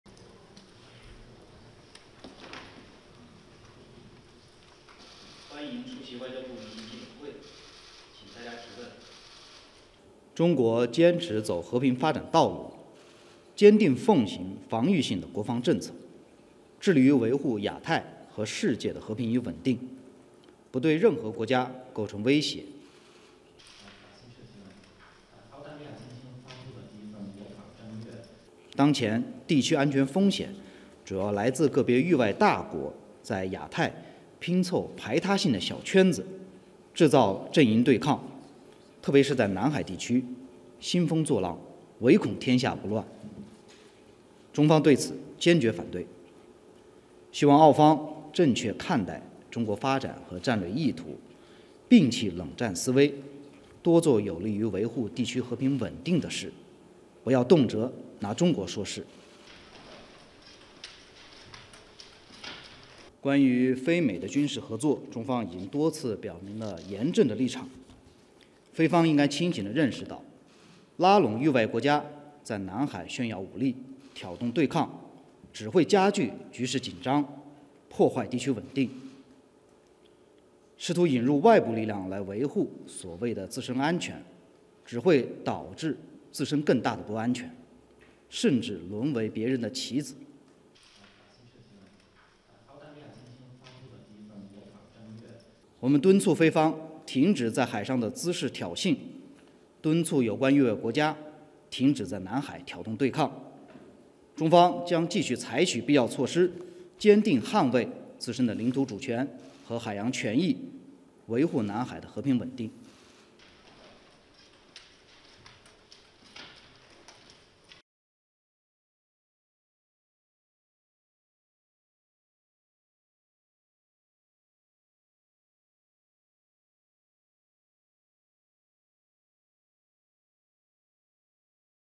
China opposes external interference in SCS; hits Australia defense strategy, PH-US drills | ANC
On The Scene: China said on Wednesday (April 17) that joint military drills by Philippine and U.S. forces in the South China Sea will only lead to "greater insecurity" for Manila. The Philippines and the U.S. are holding their annual "Balikatan" or "shoulder-to-shoulder" joint drills starting next week from April 22 to May 10, during which they will simulate retaking enemy-occupied islands in areas facing Taiwan and the South China Sea. Lin also warned that the Philippines should be "sober enough to realize" that bringing in extra-territorial countries to show off their force in the South China Sea and provoke confrontation will only aggravate tensions and undermine regional stability.